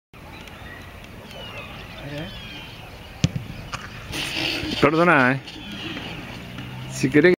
Jeronimo Pourtau
En La Primera de Estudiantes practicando penales año 2018